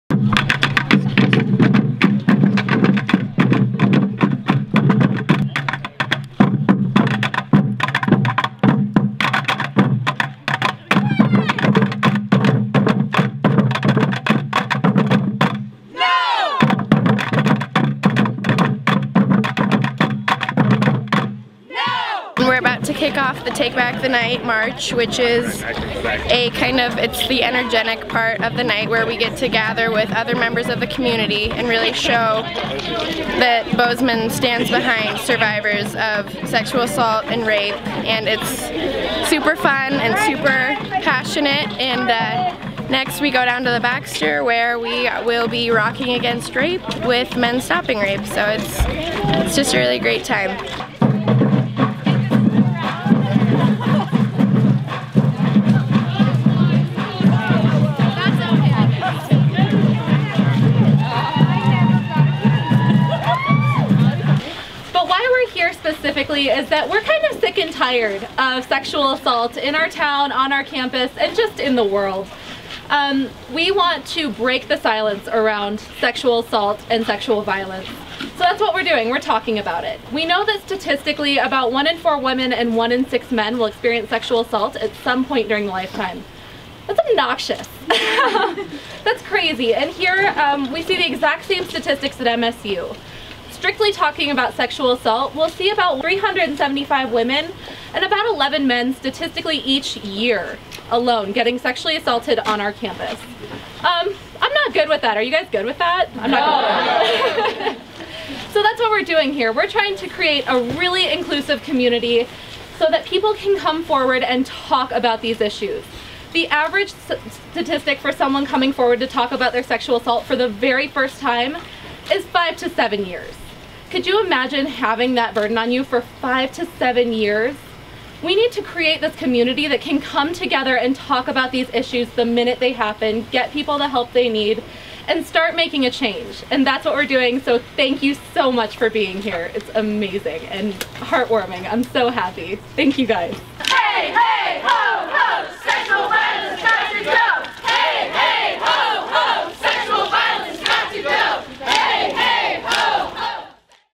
Take Back the Night
1 out of 3 women and 1 out of every 6 men have been beaten, forced into sex, or otherwise abused in their lifetime by someone they know.  Less than 50% are ever reported to the police. The Survivor Speak-Out, Take Back the Night March, and Rock Against Rape events provide the Bozeman community with the opportunity to come together, share stories, and raise voices against violence. The Exponent was at hand to follow the Take Back the Night March.